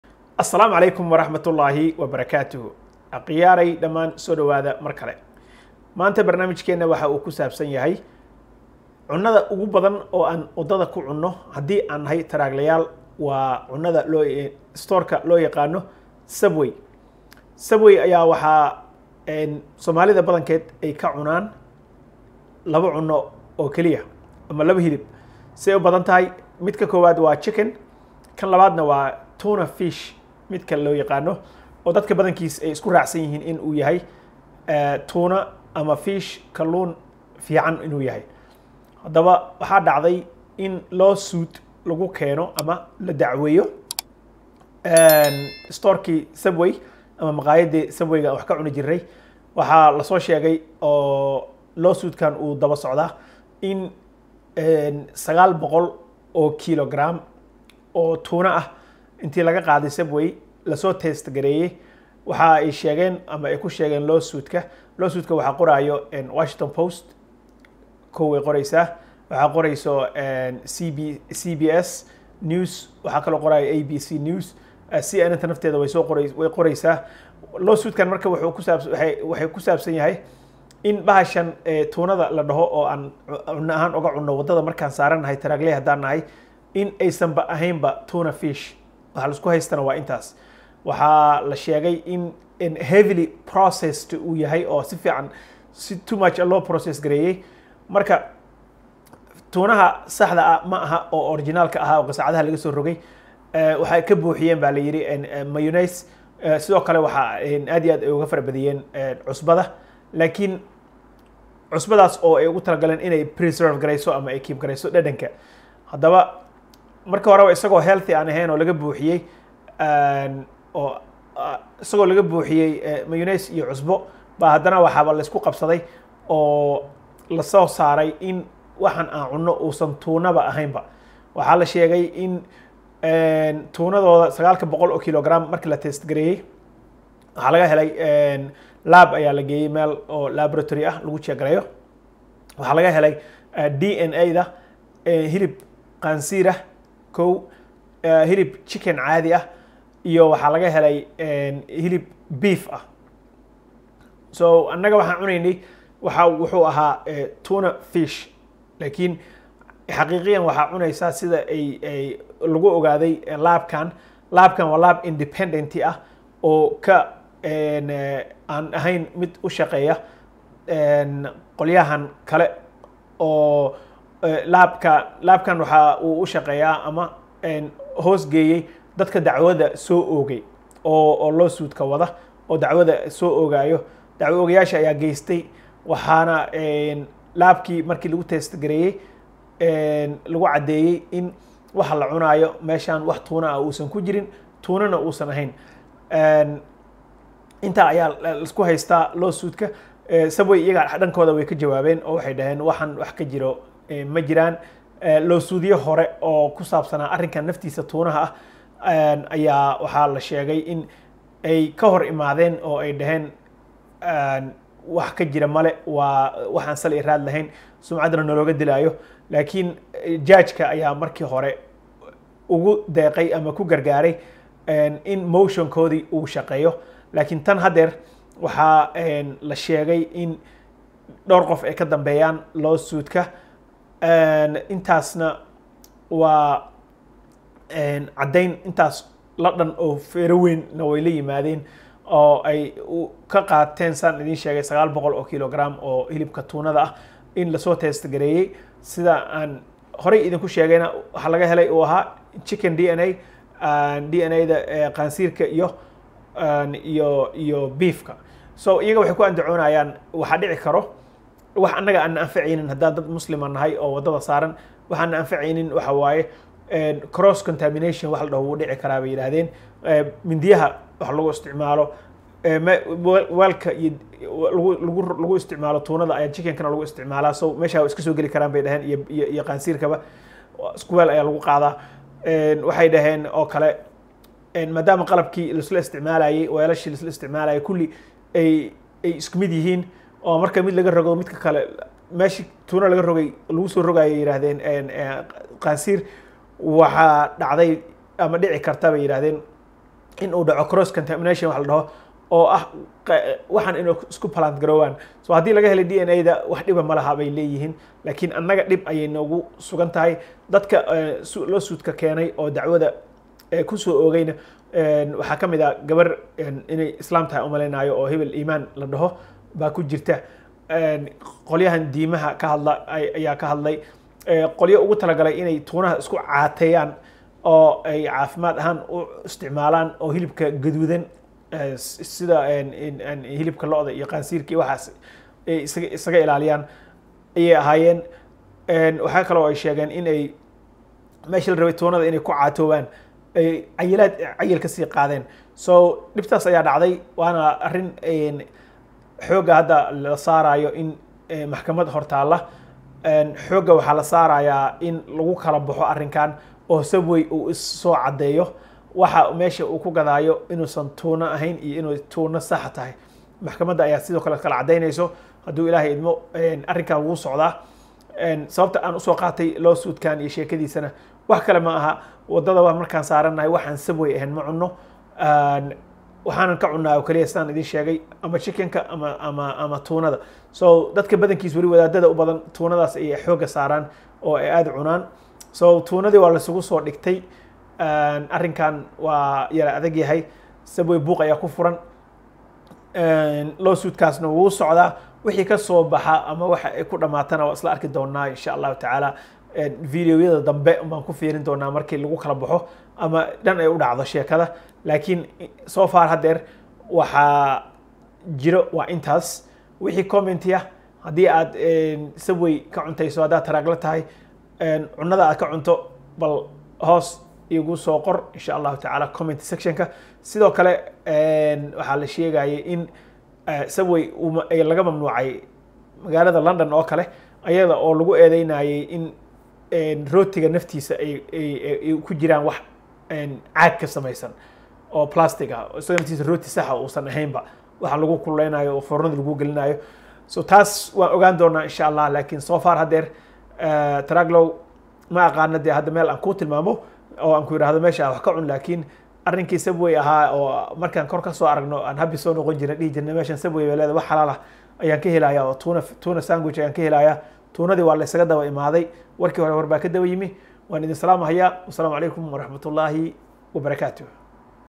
Maqaayada Subway oo dacwad lagu soo oogay.
Waa video kusaabsan dacwad ka dhan ah maqaayada Subway. Waxaa lasheegay in tuna fish ka uusan aheyn tuna fish sax ah.